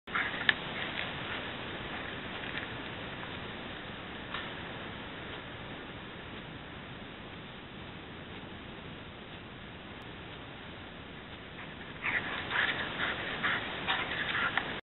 Dog playing fetch
My tennis-ball-crazy lab Jersey playing fetch our the back yard in Macomb, IL.